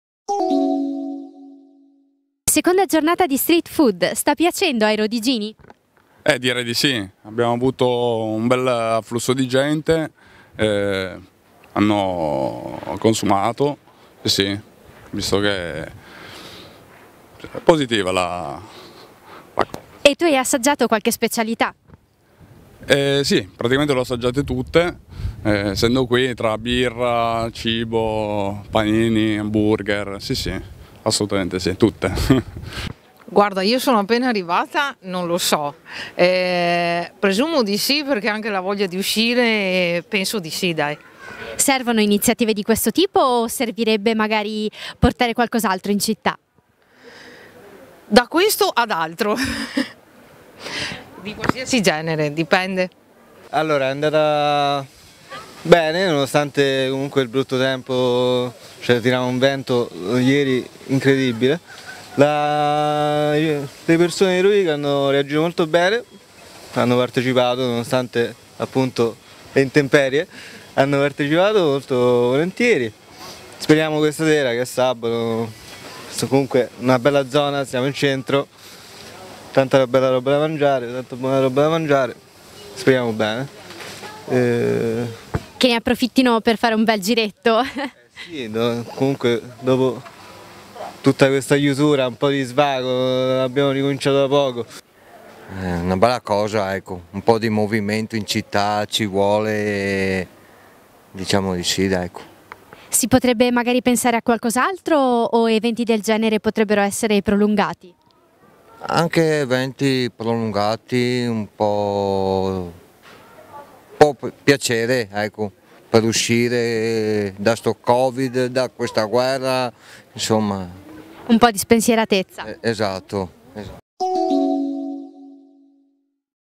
La grande festa dello Street Food a Rovigo: una manifestazione che sa davvero tanto di ripartenza
ROVIGO - Un fine settimana all'insegna dello Street Food, in centro a Rovigo. E i Rodigini sembrano gradire!